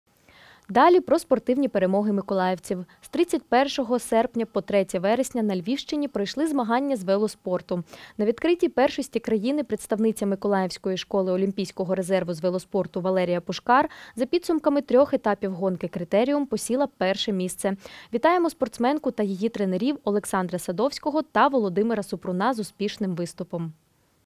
🚴🏼‍♂️ ВАЛЕРІЯ ПУШКАР здобула І місце на національних змаганнях з ВЕЛОСПОРТУ
З 31 серпня по 3 вересня на Львівщині пройшли змагання з велоспорту. На відкритій першості країни представниця миколаївської школи олімпійського резерву з велоспорту Валерія Пушкар за підсумками трьох етапів гонки «Критеріум» посіла І місце.